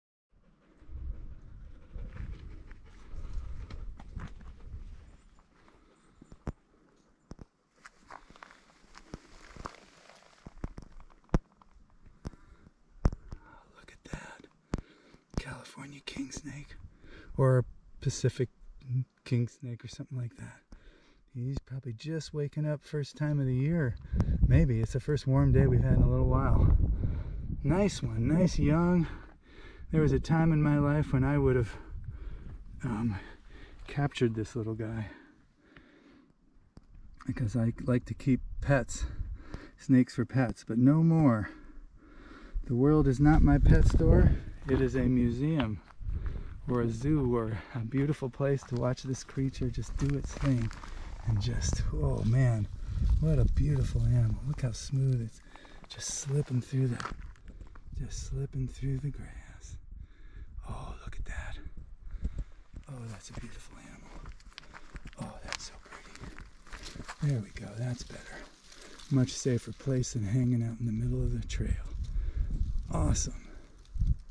Kingsnake!~Gene Burnett
On a hike with my wife and father-in-law I finally got to see a living kingsnake in the wild! I believe it's a California Mountain Kingsnake. This is on the Wolf Gap trail near Jacksonville Oregon. What a beautiful creature.